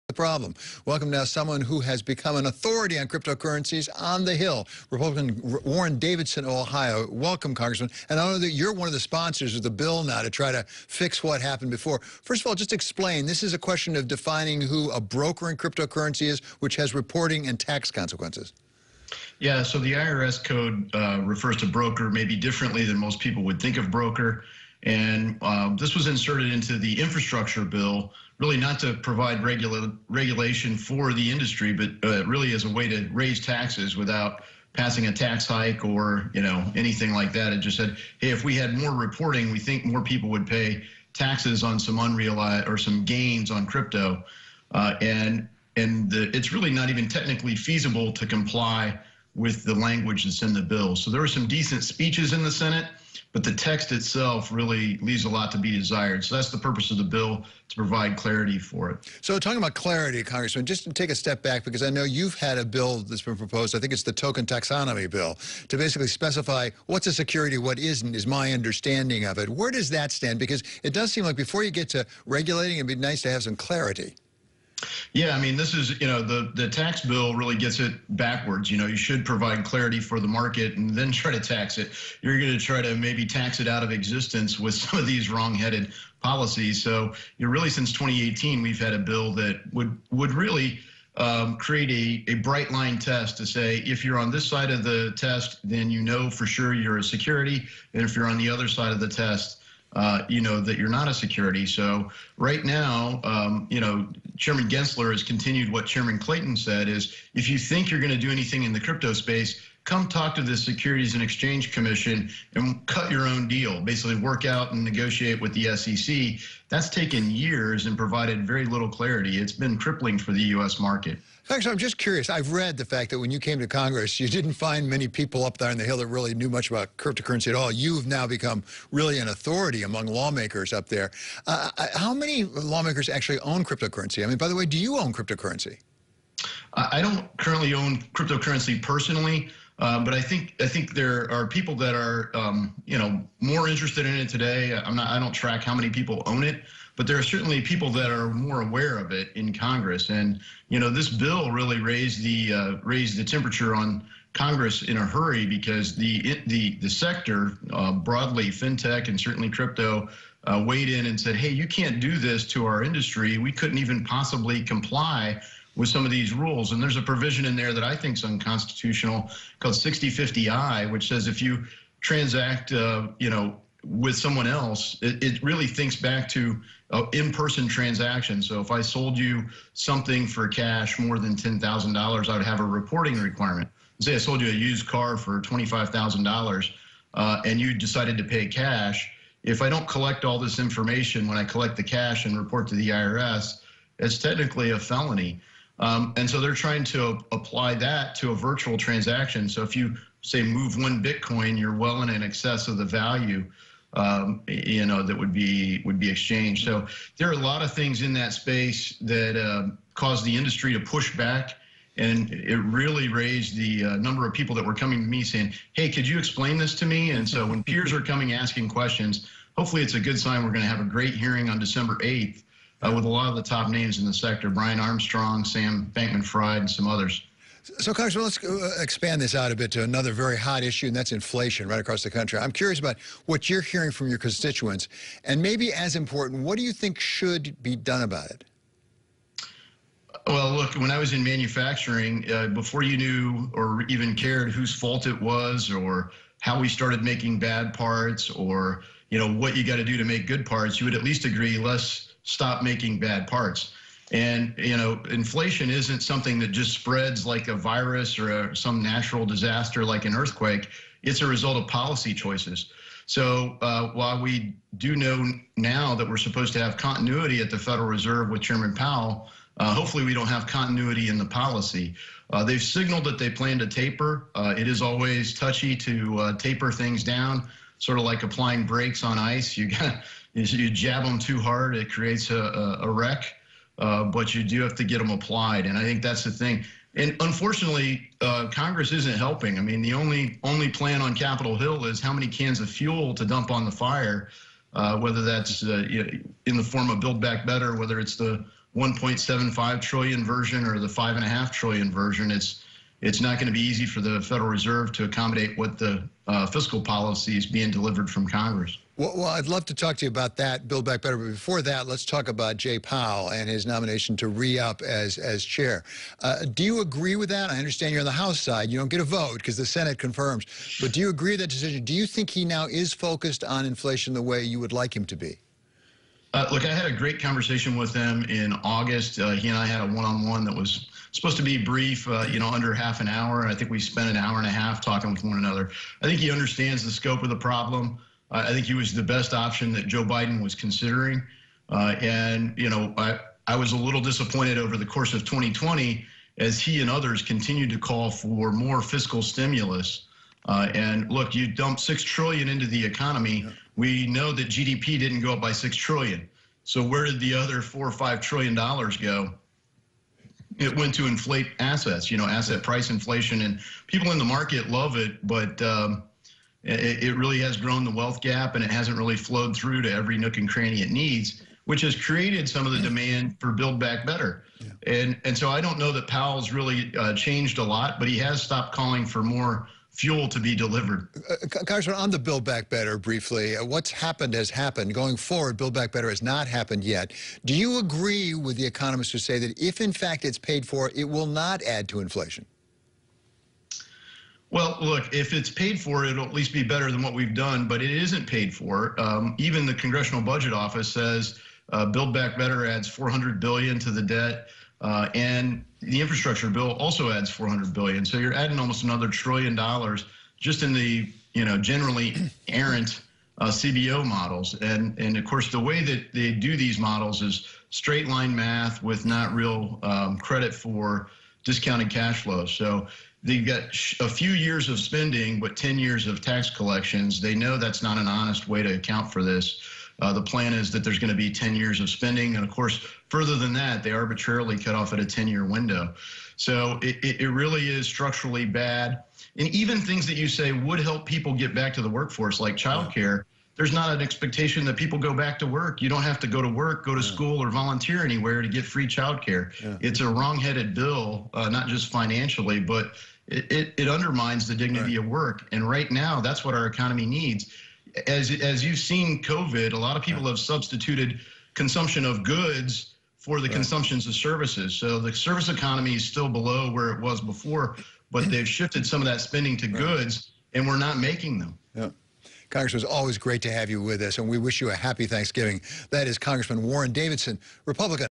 Davidson talks Inflation, Crypto Taxes & Build Back Better
Ohio Congressman Warren Davidson (R) talks about the flaws in the infrastructure bill that threaten the crypto industry, the policy choices that lead to inflation, and Congress's spending addiction.